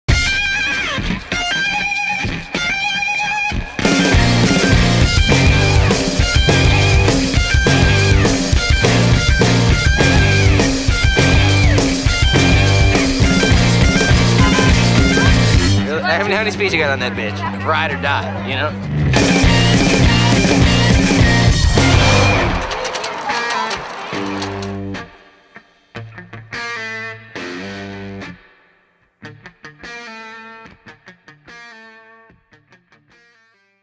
Hilliard Davidson Senior Bike Trip
2008 Seniors at Hilliard Davidson High School take a little bike trip down Davidson Road.